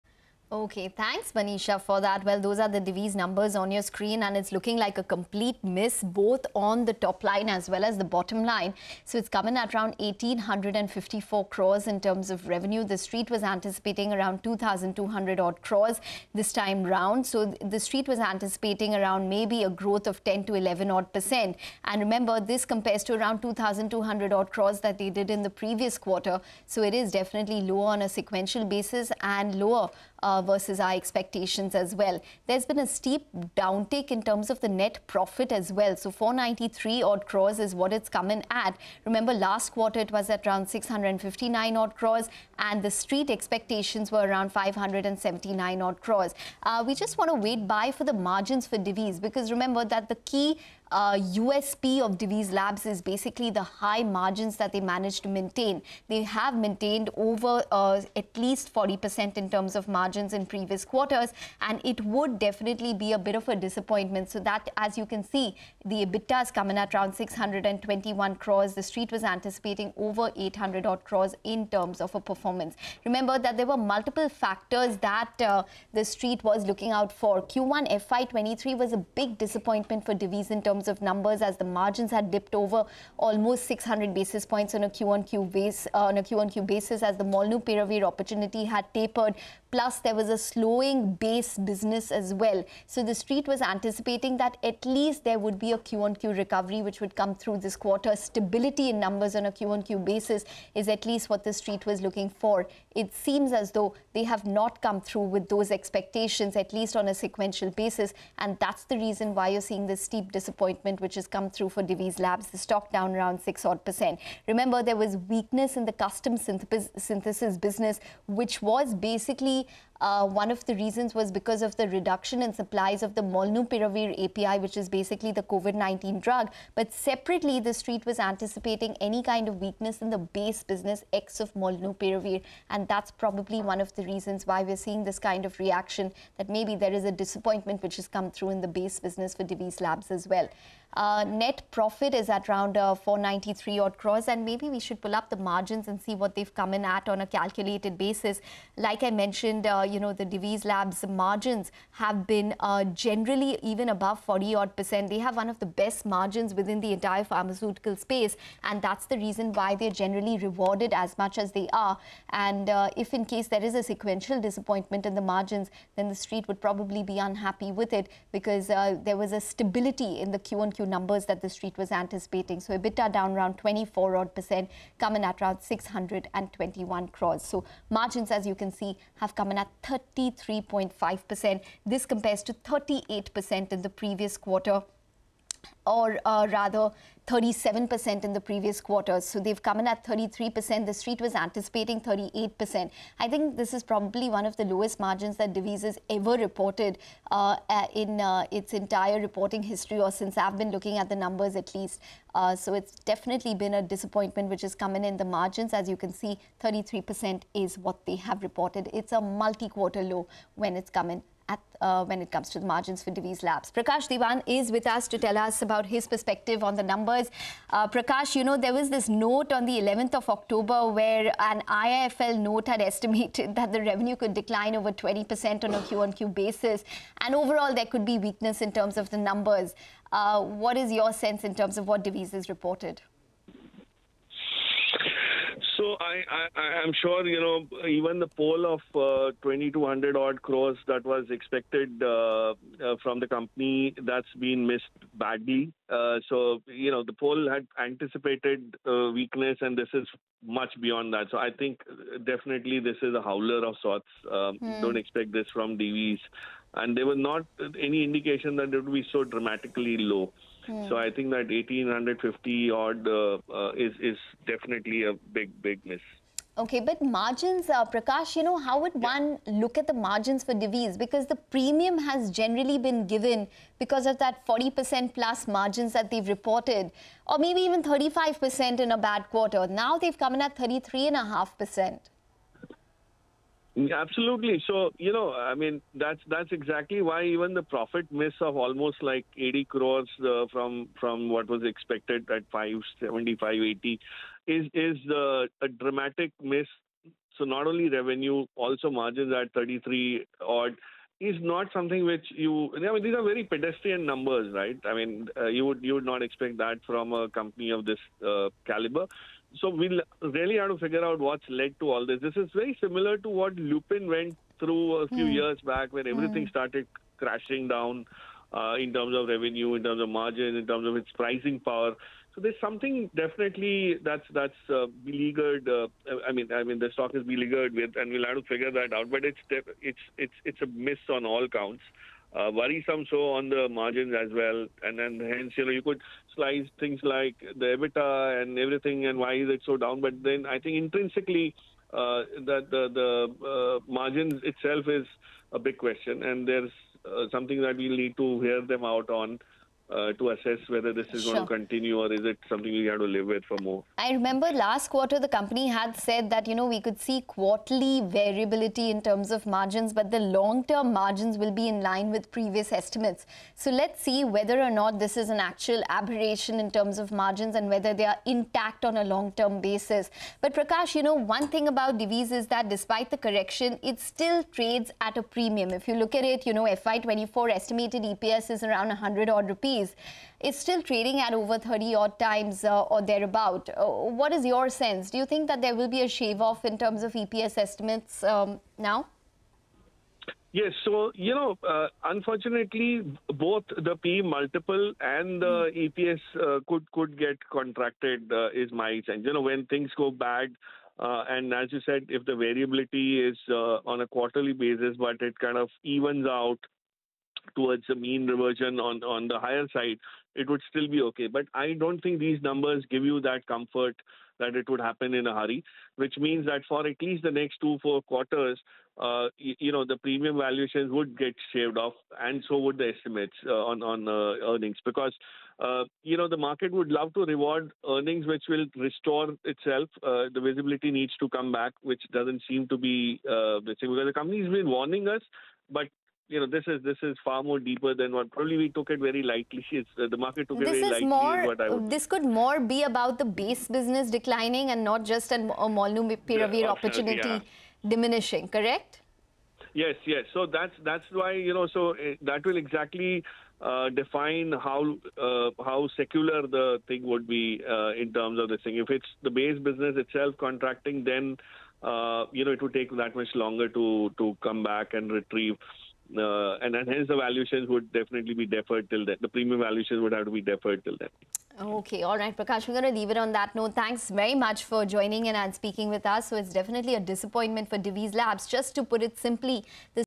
Divi's Labs Q2 Results: Profit Falls 19% YoY To ₹494 Crore: Prakash Diwan Exclusive | CNBC-TV18
Divi's Laboratories on Monday reported an 18.5% year-on-year (YoY) drop in its September quarter consolidated profit after tax to Rs 494 crore. The Hyderabad-based drugmaker disappointed the Street which was estimating a profit of Rs 630 crore. The company's total revenue dipped 3.6% YoY to Rs 1,935 crore for the quarter ended 30th September as against a consolidated total income of Rs 2,007 crore in the corresponding quarter of the previous year. On a standalone basis, the company's PAT was at Rs 487 crore as against Rs 606 crore seen in the year-ago period.

Market expert Prakash Diwan shares his insights on Divi's Laboratories' Q2 results. Listen in.

n18oc_business